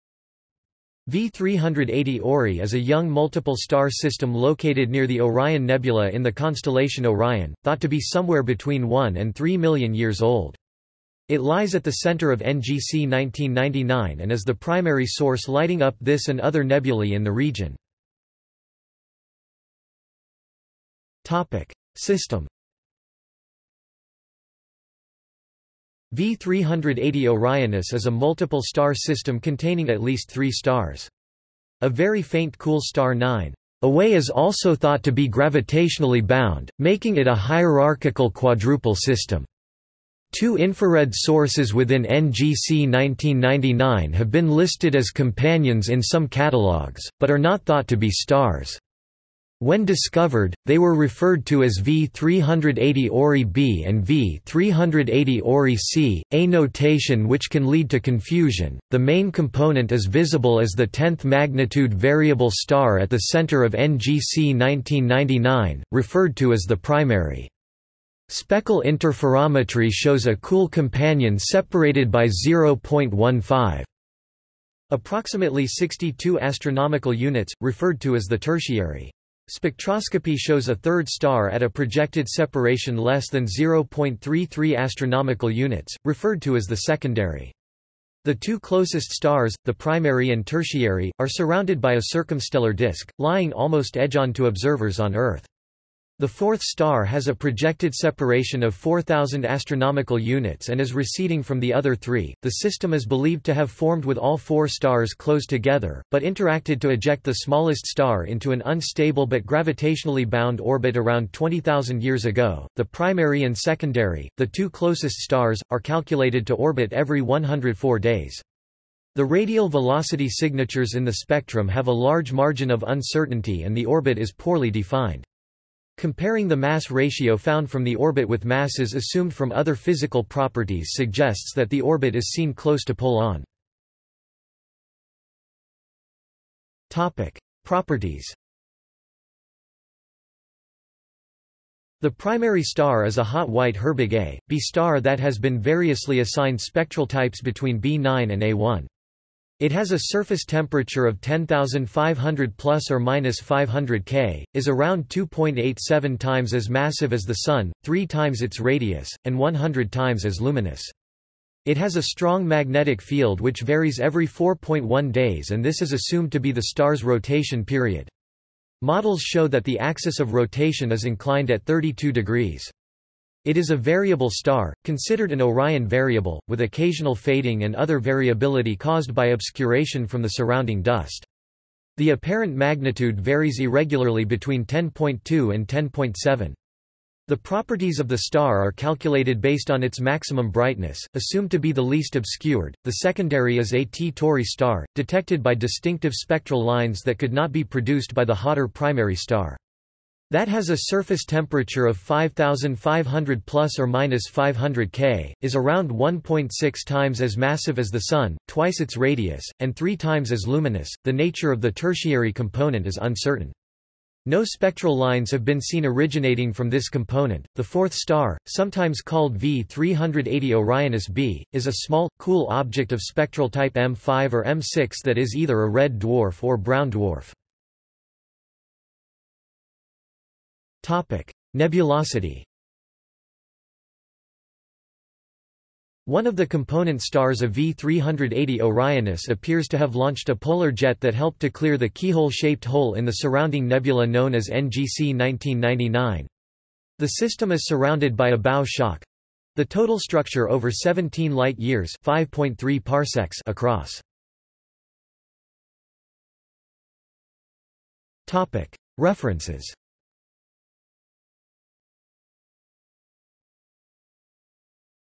V380 Orionis | Wikipedia audio article
SUMMARY
V380 Ori  is a young multiple star system located near the Orion Nebula in the constellation Orion, thought to be somewhere between 1 and 3 million years old.  It lies at the centre of NGC 1999 and is the primary source lighting up this and other nebulae in the region.